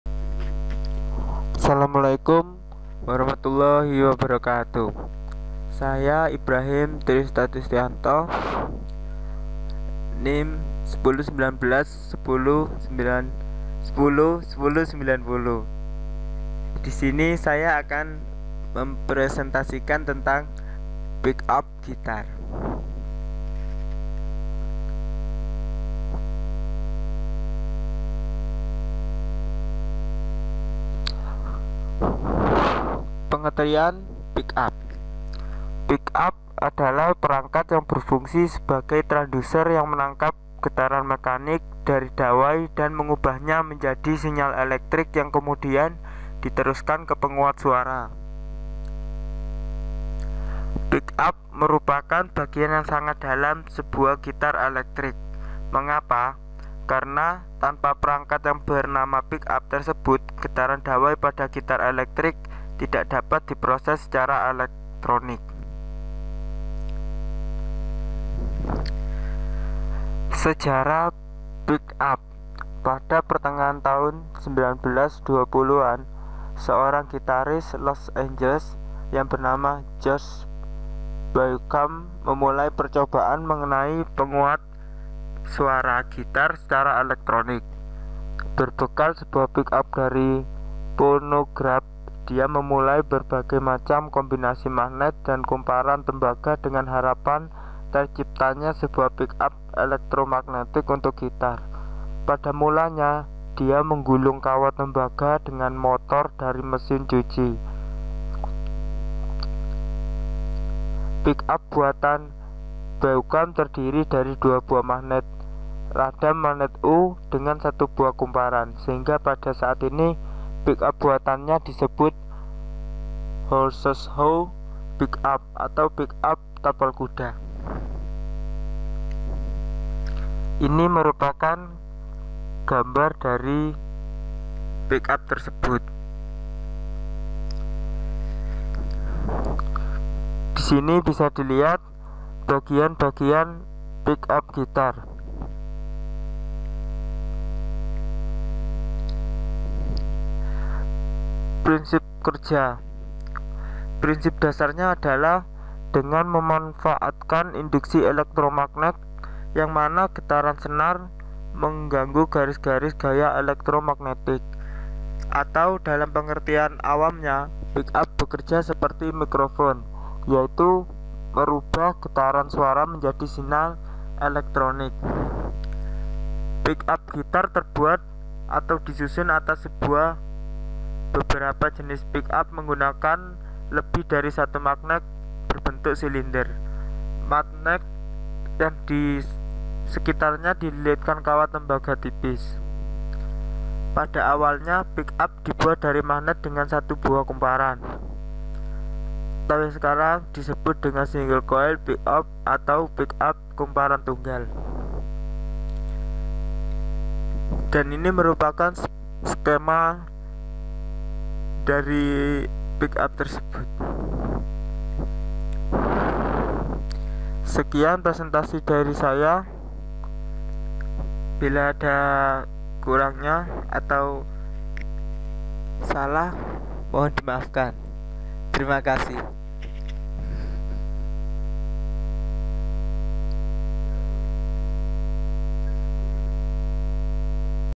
Macam-macam Transduser 045 (Pickup Guitar) oleh Ibrahim Tri Statistianto
Video presentasi mahasiswa tentang macam-macam transduser dan penjelasan tentang bagaimana cara kerjanya. Dipresentasikan dalam matakuliah pengukuran teknik pada Teknik Mesin Universitas Jember.. Dosen pengampu : Aris Zainul Muttaqin